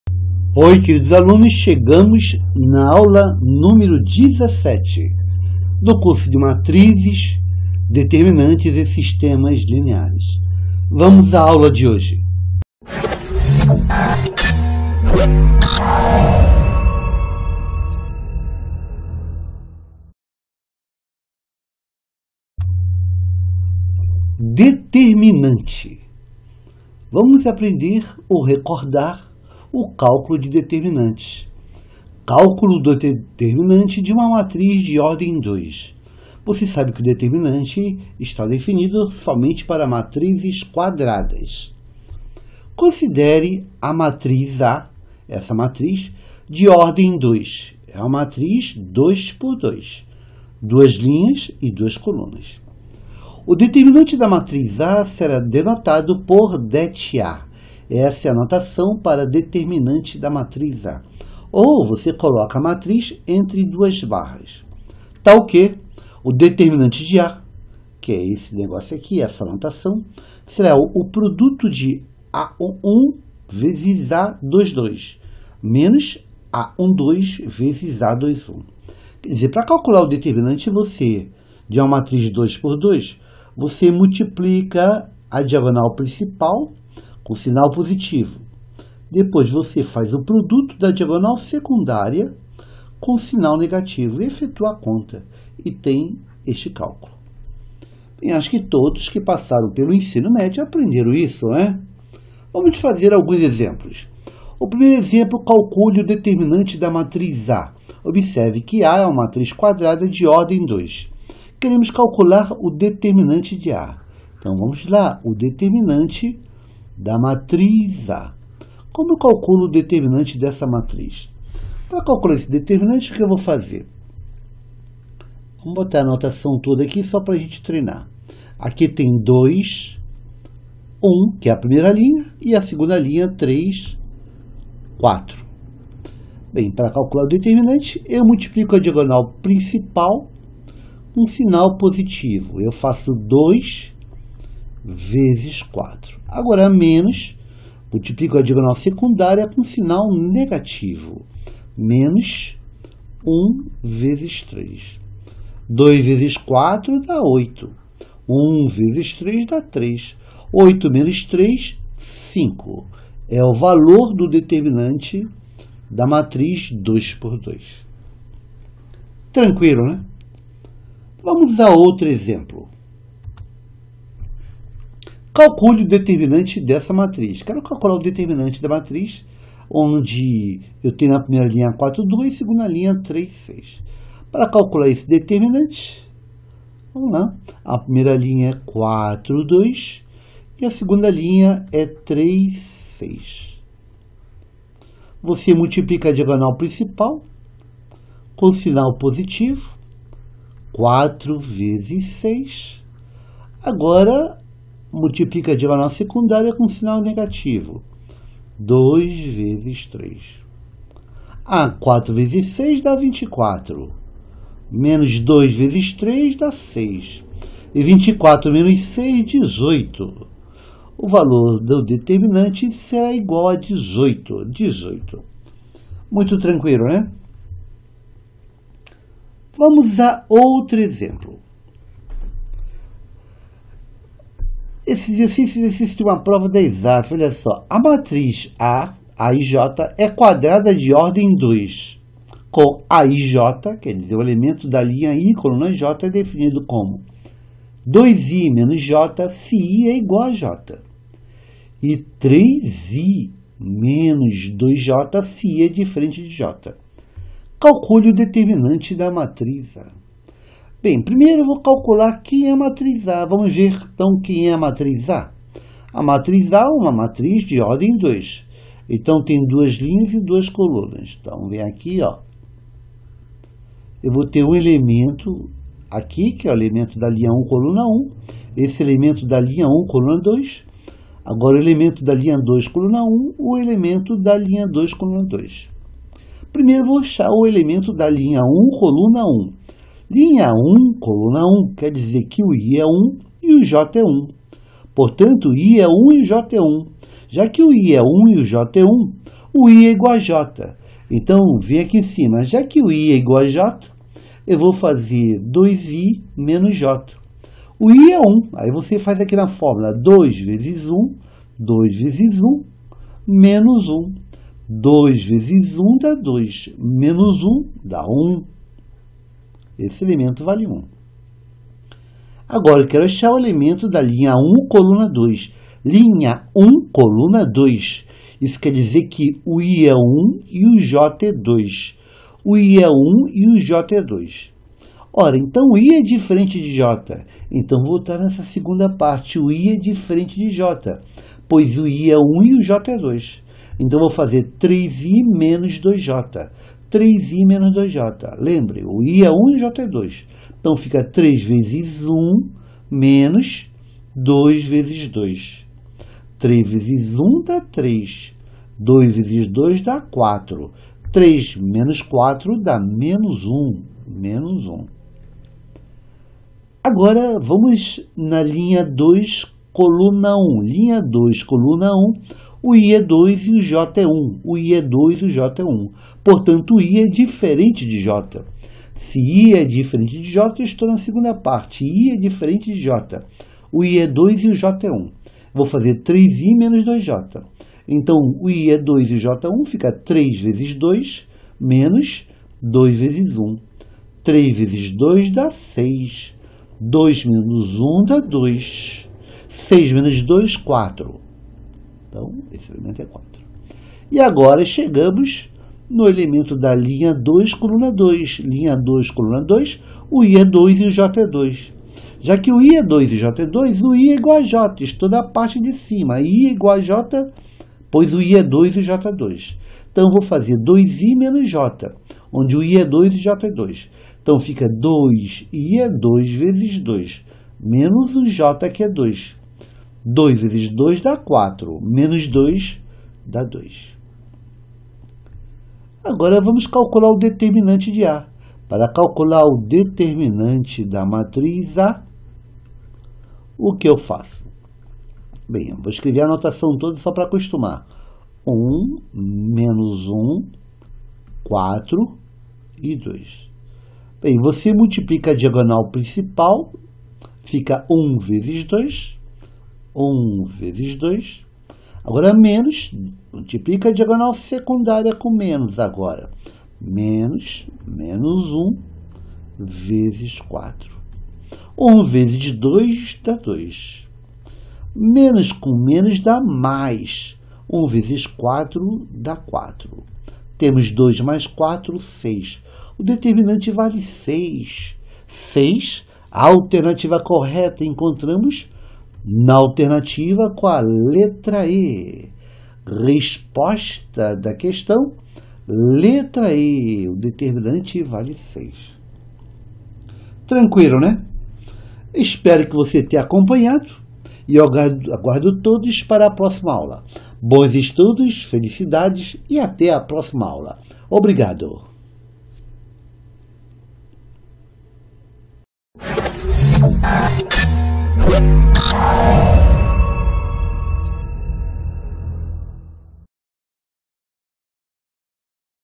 Aula 17 - Matrizes, Determinantes e Sistemas lineares - Determinantes
Baixe o material de apoio gratuito do curso de Matrizes e Determinantes em PDF no

Entre em contato com o professor Joselias e conheça os também os novos cursos online do professor Joselias.